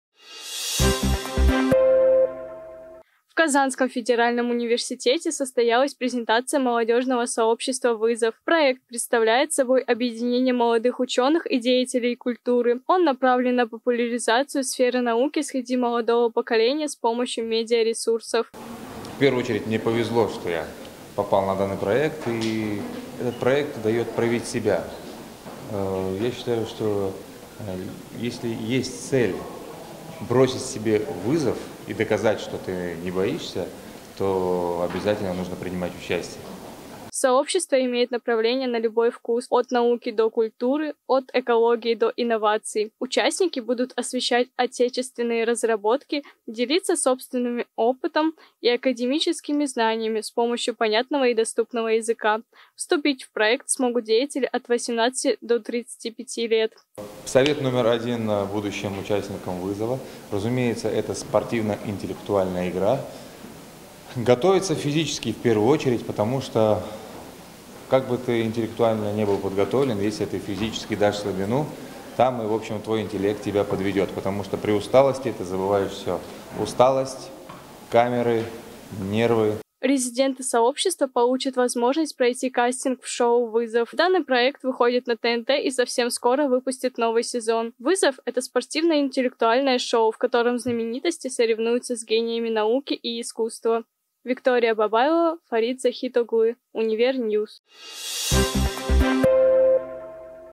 НАУКУ В МАССЫ
Молодежный проект "Вызов" выступит в Казанском федеральном университете с презентацией своей идеи по развитию научно-культурной сферы с помощью медиапространства. Подробности в сюжете